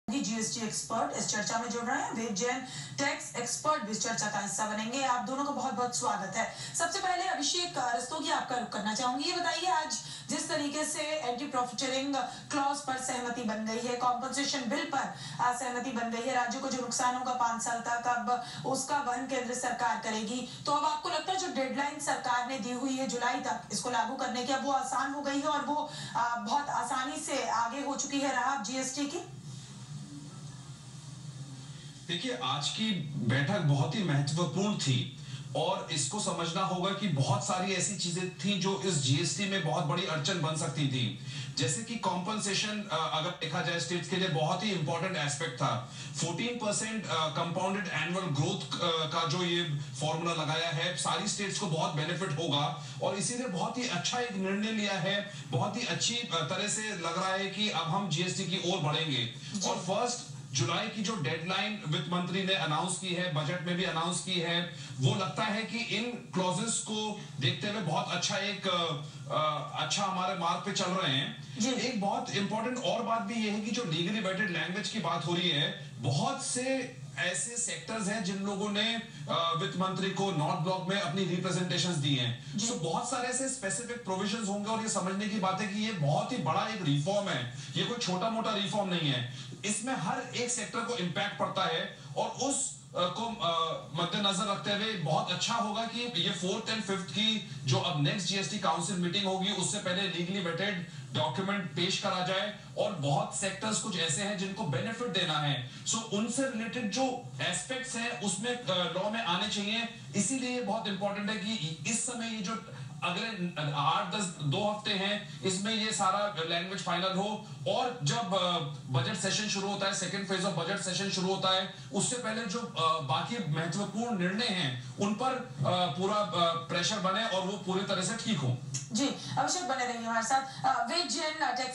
Abhishek A Rastogi on Zee business after 10th GST council meeting
Abhishek A Rastogi, partner of Khaitan & Co, expresses his views after the 10th GST council meeting at Udaipur.